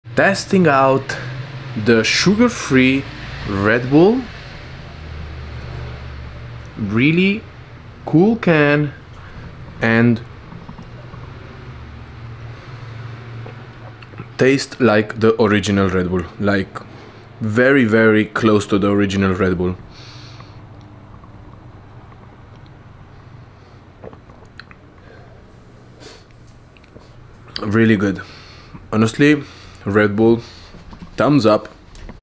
Savor the Flavor without the Sugar Rush: Red Bull Sugar-Free - Honest Review and Taste Test!
🌟 Dive into the world of energy without the sugar overload! Join us as we embark on a flavorful journey with Red Bull Sugar-Free in this honest taste test and review. Discover the perfect blend of bold taste and sustained energy, minus the guilt of added sugars. Whether you're looking for a pick-me-up at work or gearing up for a workout, Red Bull Sugar-Free might be your ideal companion. 💙